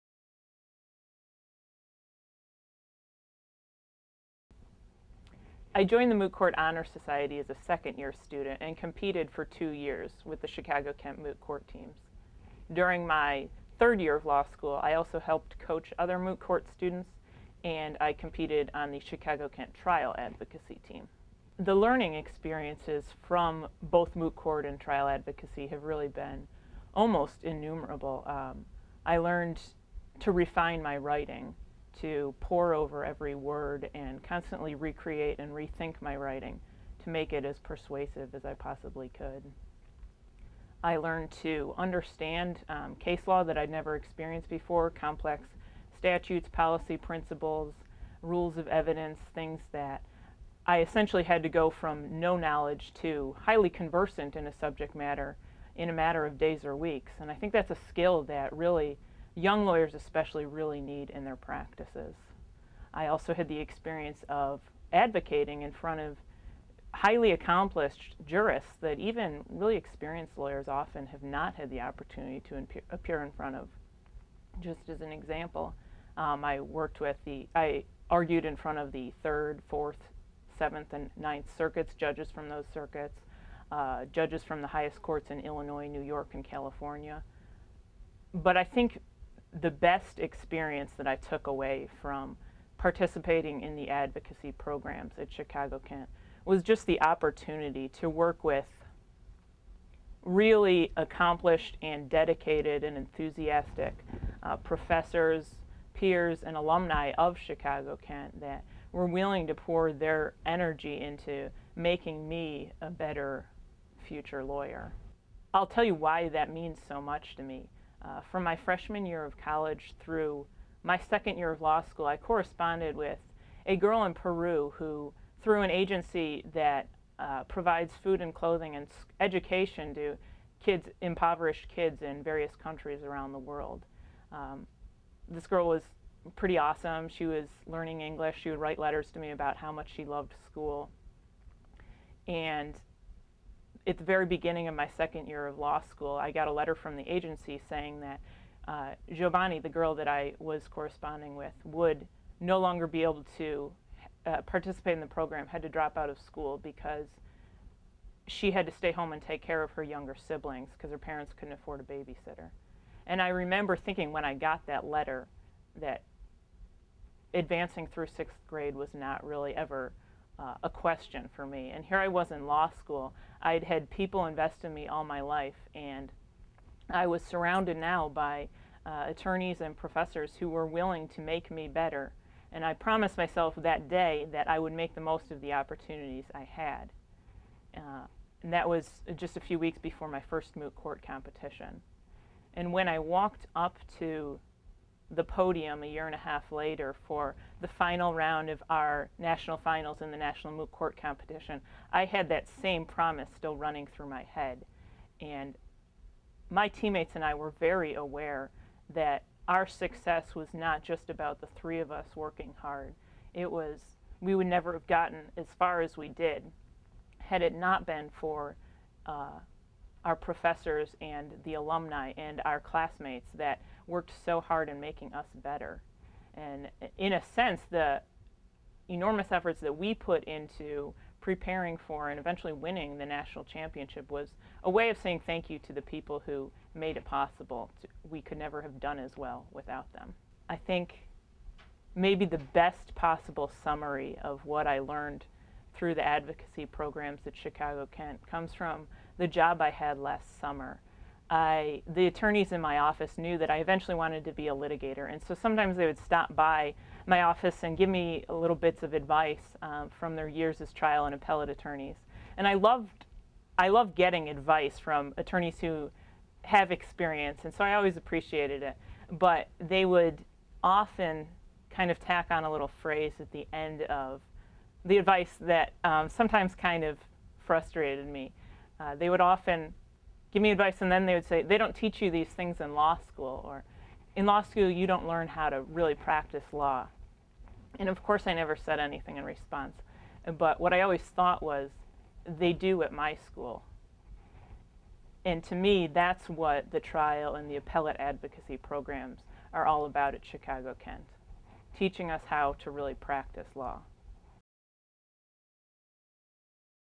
Advocacy Champions: Rachel Moran '08
This interview with alumna Rachel Moran '08 is part of Chicago-Kent College of Law's Advocacy Champions series that features national champions of trial advocacy and appellate advocacy competitions.